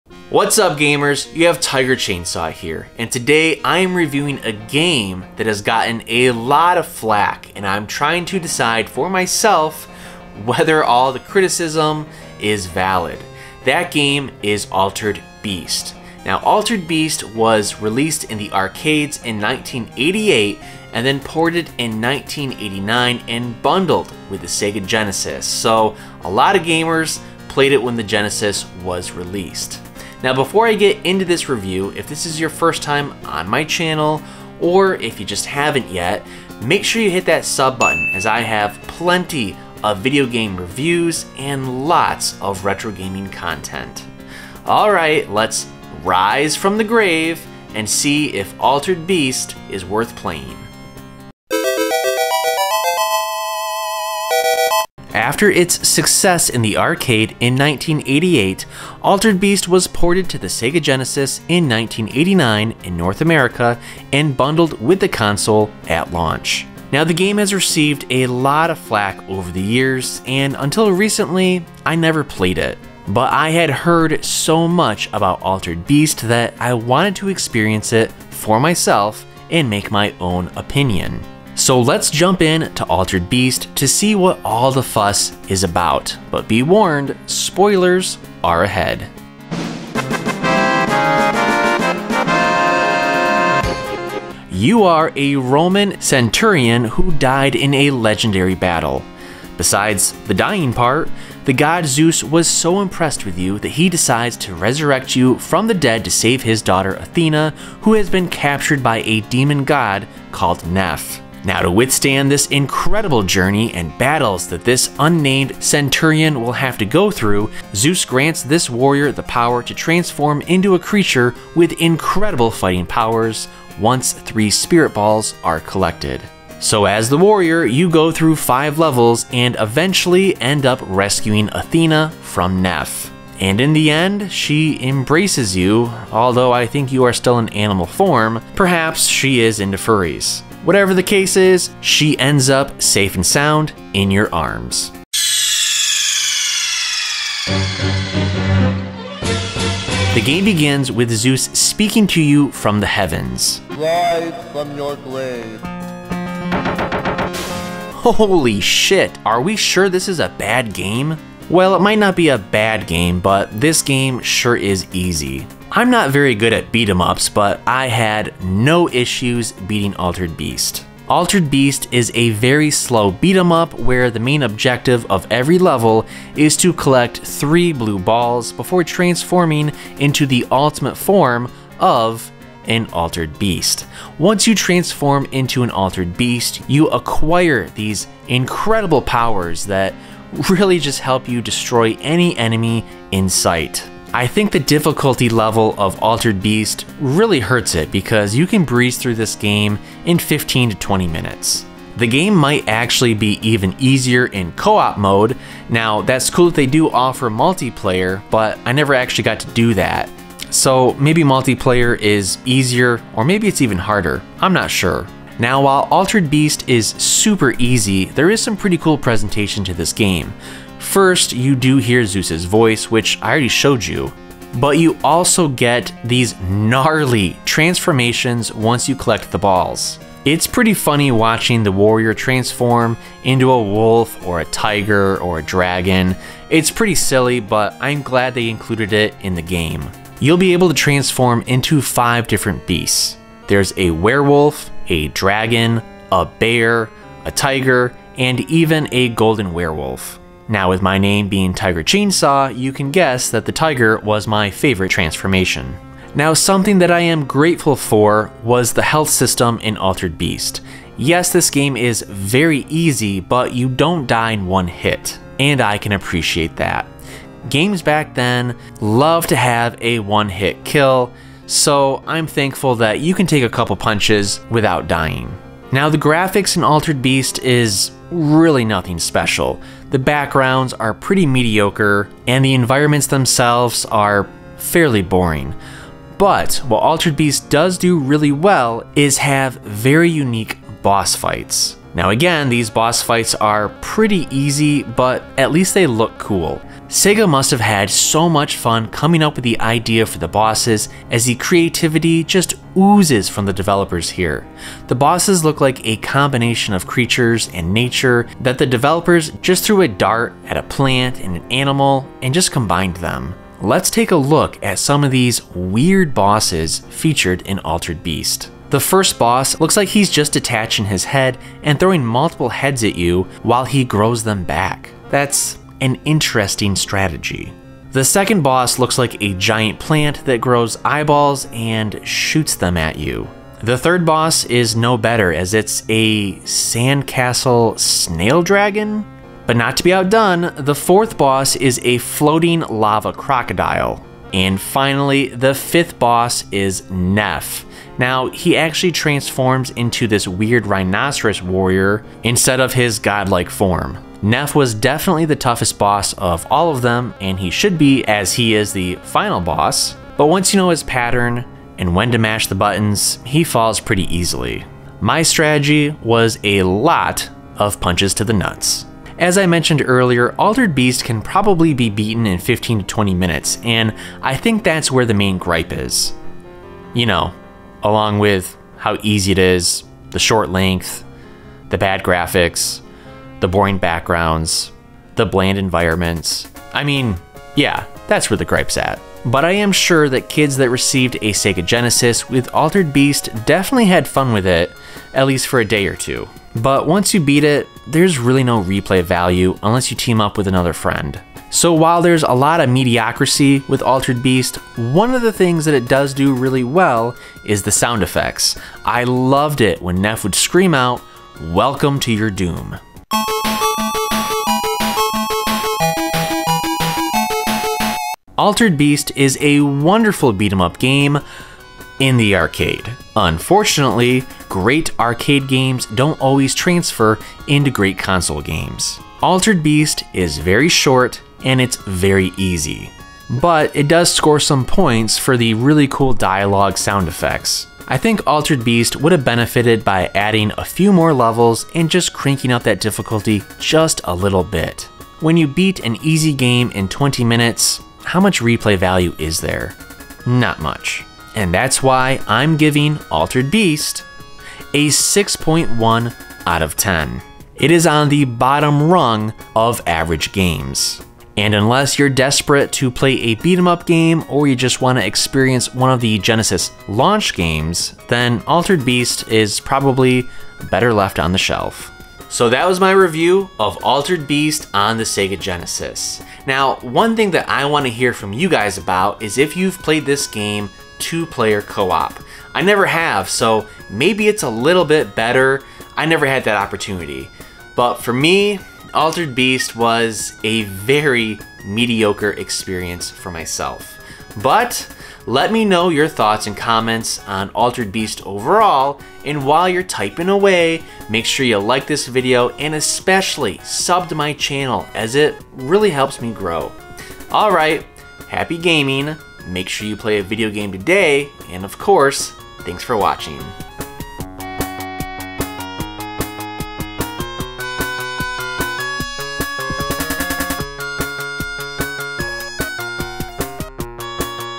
Altered Beast Review | SLOW BORING AND SHORT
After its success in the arcade in 1988, Altered Beast was ported to the Sega Genesis in 1989 in North America and bundled with the console at launch. The game has received a lot of flak over the years, and until recently, I have never played it. It wasn’t until a few months ago that I decided to do an Altered Beast review and see what all the fuss was.